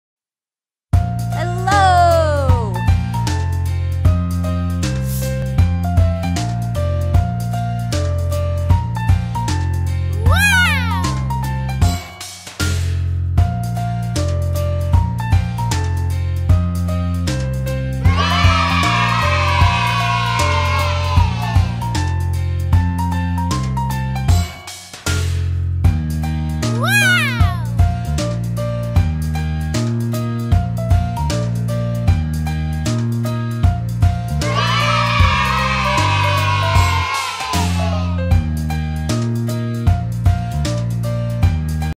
The Smurfs - Super Fun Cartoon Puzzle for Kids and Parents
A very funny puzzle for kids with Smurfs, very smart and kind people to make you laugh and make you smarter. Watch, have fun and learn to develop the thinking of our kids together.